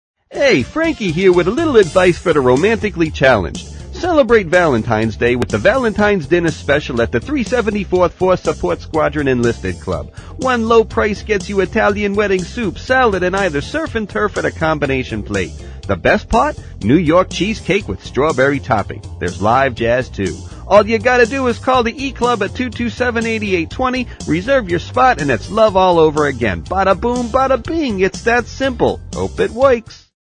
Valentine's Day Radio Spot
This radio spot is one in a series of radio spots using the fast talking Italian character "Frankie" who gives advice to romance-challenged husbands.